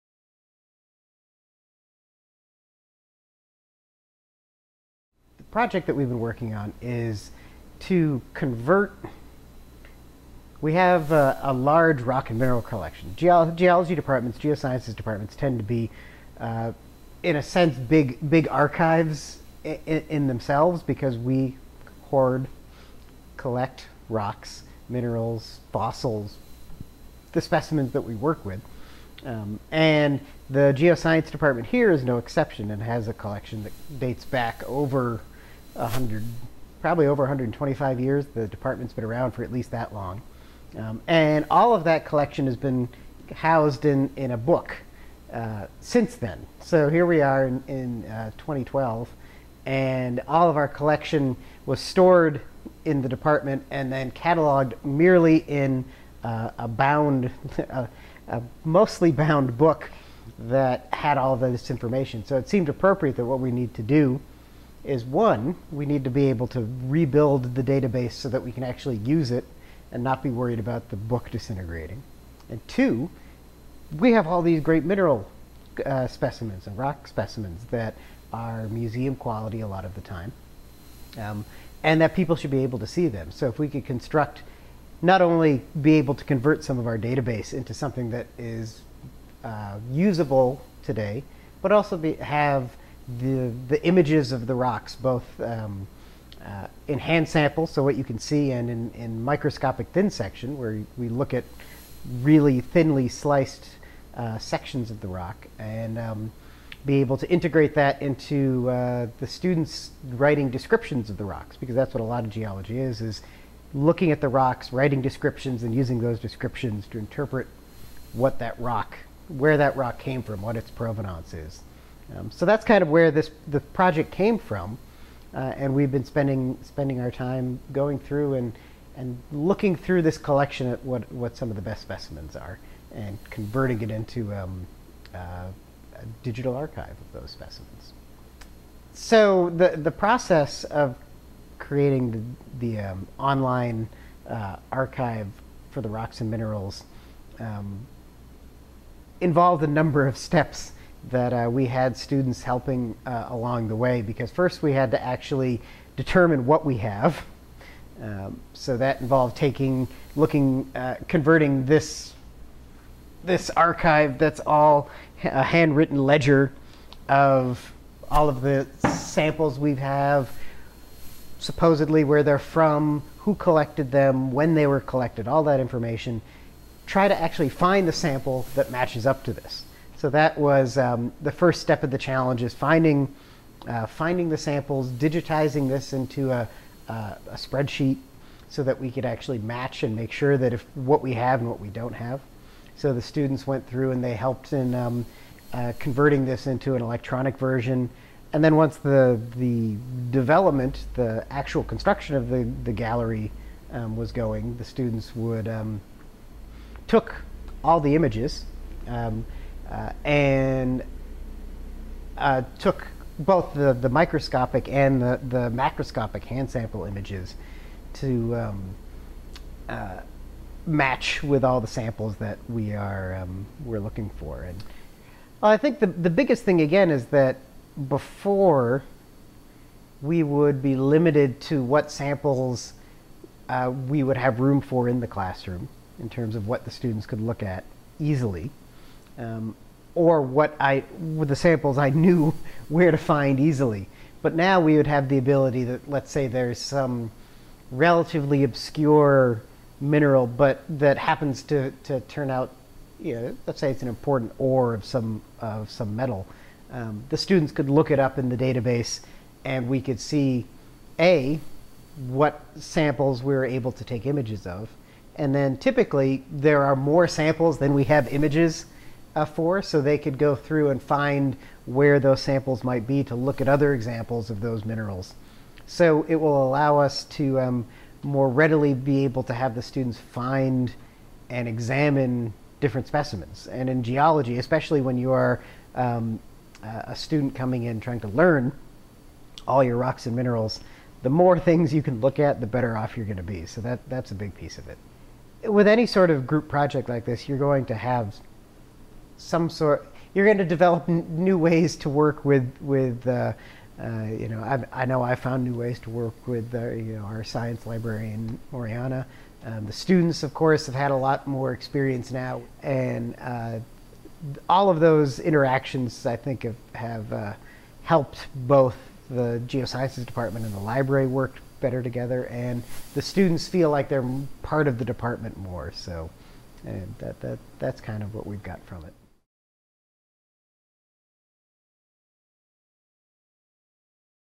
NGL Grant: Denison Virtual Earth Material Gallery
The Department of Geosciences at Denison University celebrated its 175th anniversary in 2011 (Mahard, 1979). Its long history is reflected in the mineral, rock and fossil collection that has been growing since the late 19th century. This virtual gallery is a sampling of the upward of 9,000 specimens from the Denison Geosciences Earth materials collection previously only cataloged in a manuscript book (Figure 1.a and b) started by Prof. Clarence L. Herrick (Figure 1.c) in the 1890s. The items in the collection are varied, from representative mineral samples (Figure 1.d - Galena, quartz, rhodochrosite), to rocks from important geologic locations. The collection is much more than a rockhound's dream, but rather an essential part of the teaching experience at the Department of Geosciences at Denison. It is used both as an example of important Earth materials, and as a source of research for students during the course of their Denison career.

This initial phase of the collection contains many of the minerals and rocks of the Geosciences collection and future work will add samples from the fossil collection as well. The collection is meant to showcase the panoply of Geology, displaying the Earth materials that make up the world around us in all their variety of form and feature.